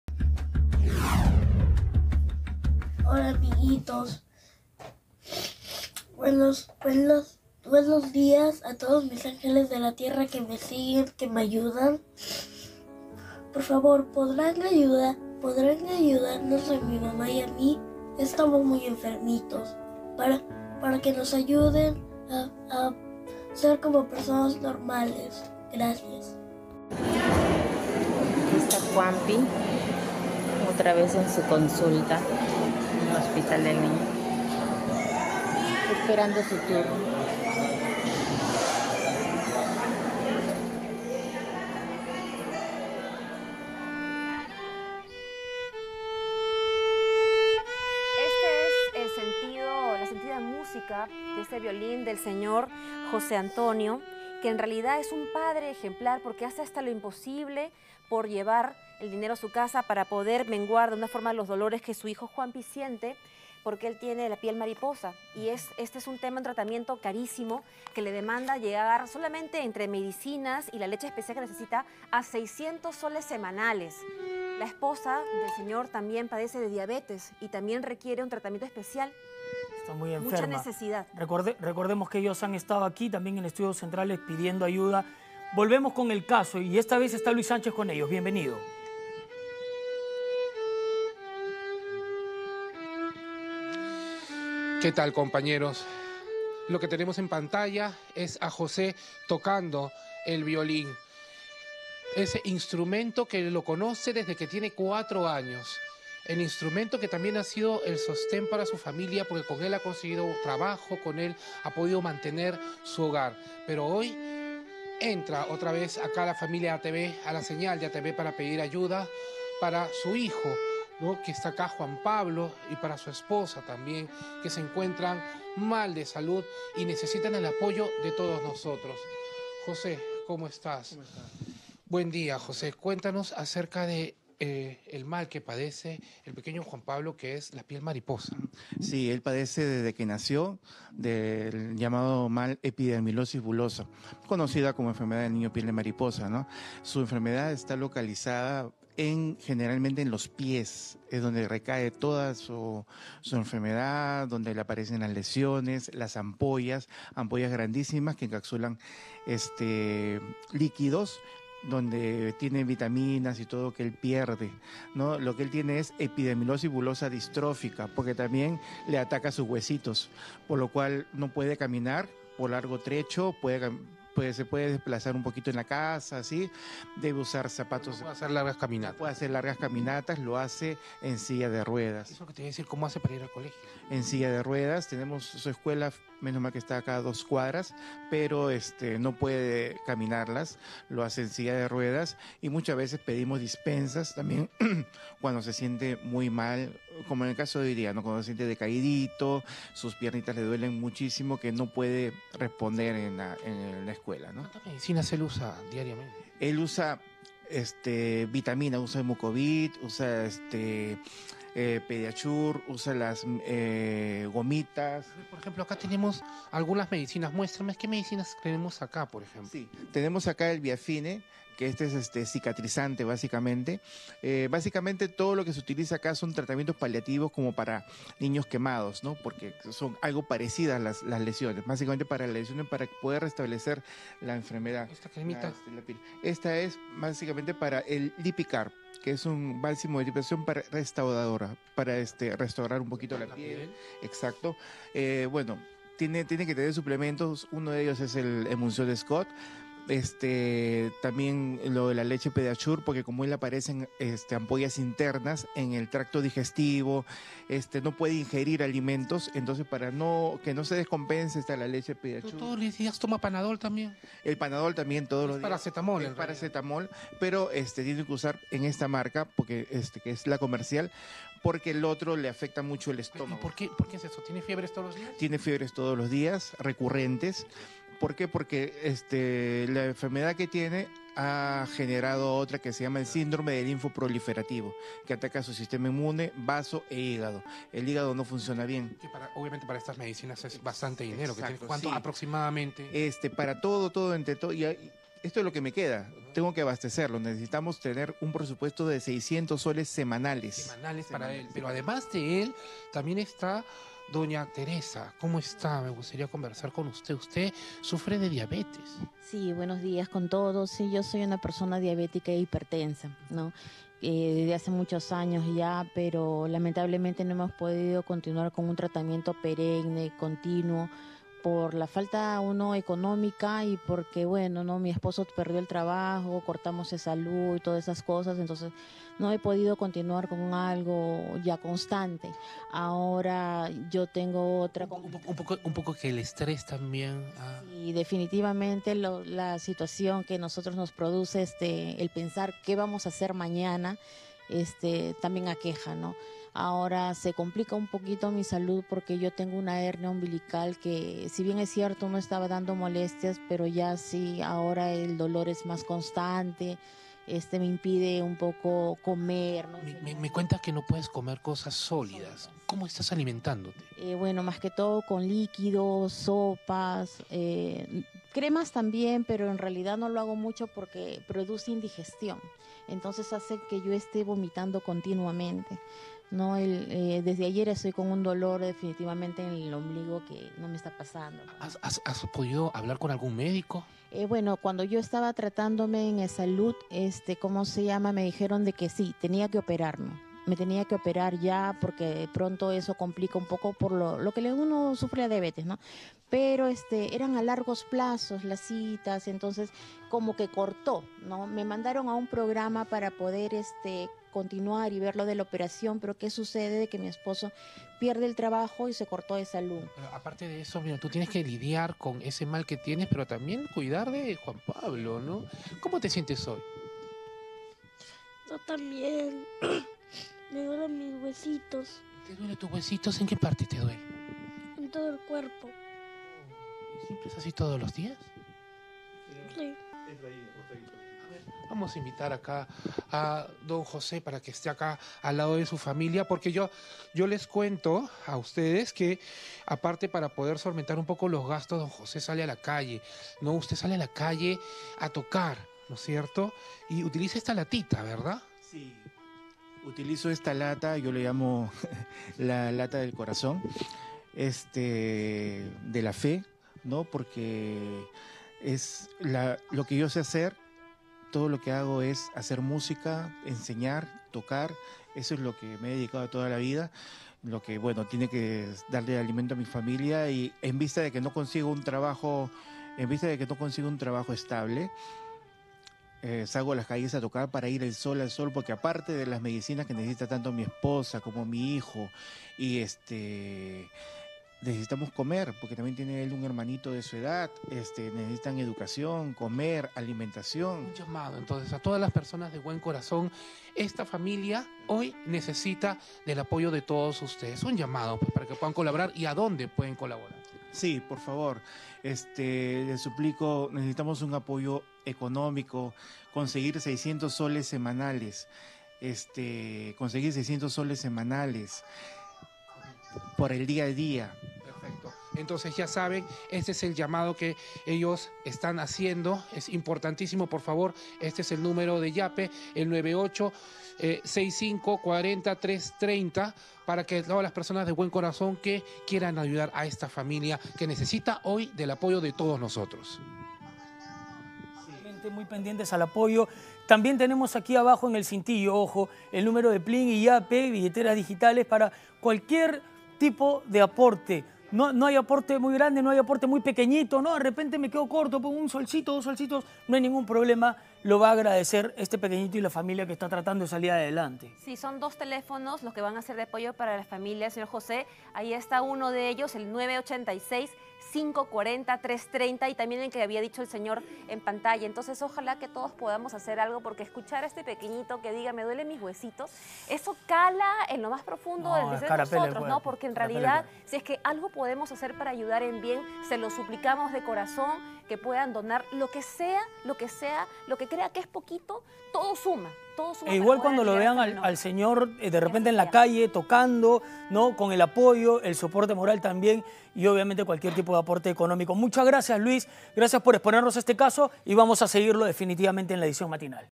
Padre de familia toca el violín en las calles y hace de todo para curar a su esposa e hijo
Un profesor de violín y su familia no están pasando por el mejor de sus momentos y, debido a esto, se ha visto obligado a tocar violín en las calles. Todo esto, no solo para mantener a sus seres más amados, sino también para costear los tratamientos de ambos.
Su hijo, por ejemplo, ha sido diagnosticado con piel mariposa y entre sus mismas medicinas y la leche especial que necesita, debe llegar a los 600 soles semanales. Por otro lado, la esposa del señor padece de diabetes y también requiere de ciertas medicinas.